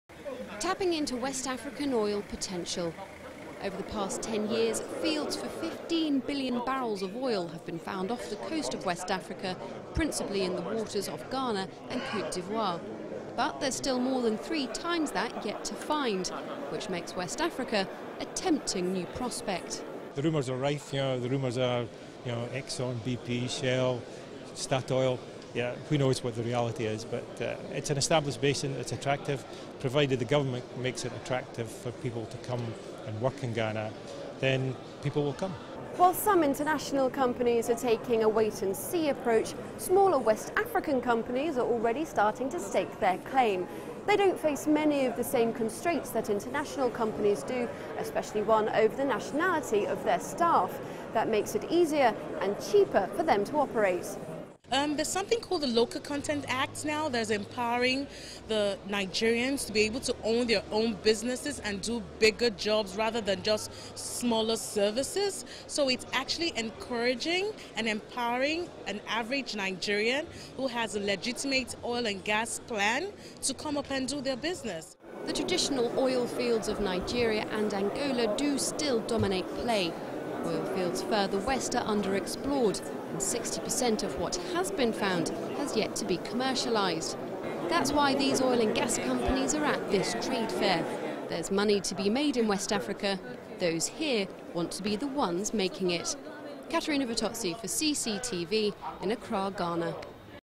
Int'l oil & gas industry leaders meet in Accra for the 18th annual Offshore Oil conference
International oil and gas industry leaders met in Ghana's capital, Accra as part of a 3-day trade fair. The 18th annual Offshore Oil conference was focused on how West Africa could fully exploit its oil and gas potential. CCTV's Katerina Vittozzi, was there.